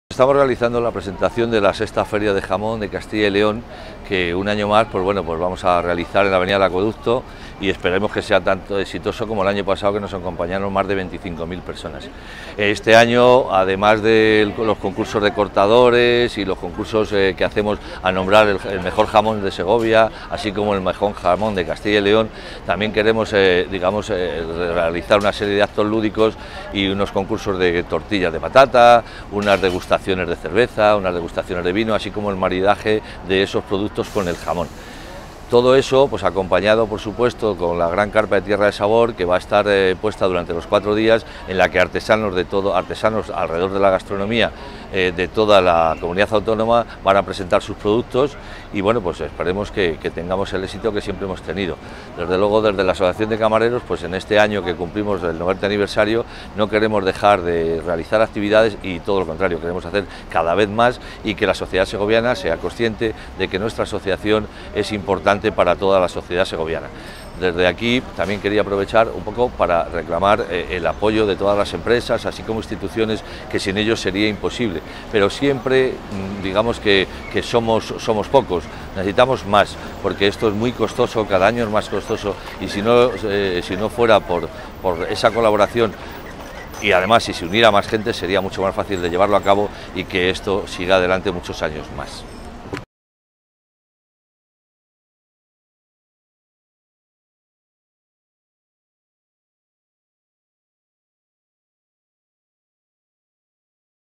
Concursos, maridajes con vino y cerveza, baile vermú y degustaciones en la VI Feria del Jamón
La VI Feria del Jamón de Castilla y León, que se celebrará del 13 al 16 de septiembre en la Avenida del Acueducto, con la gran carpa de Tierra de Sabor, vendrá acompañada, además de los tradicionales concursos de corte y jamones, de una prueba de tortillas, masterclasses de maridaje de cervezas y vinos, lecciones de afilado de cuchillos, baile vermú y, cómo no, la esperada degustación de jamón. “Esperemos que sea tan exitosa como el año pasado, que nos acompañaron más de 25.000 personas”, ha deseado el presidente de la Asociación de Camareros de Segovia, Javier Bermejo Soto, en el año de la celebración de su 90º aniversario. "Nuestra asociación es importante para toda la sociedad segoviana. Queremos reclamar el apoyo de todas las empresa e instituciones para que esto siga adelante muchos años más", ha añadido.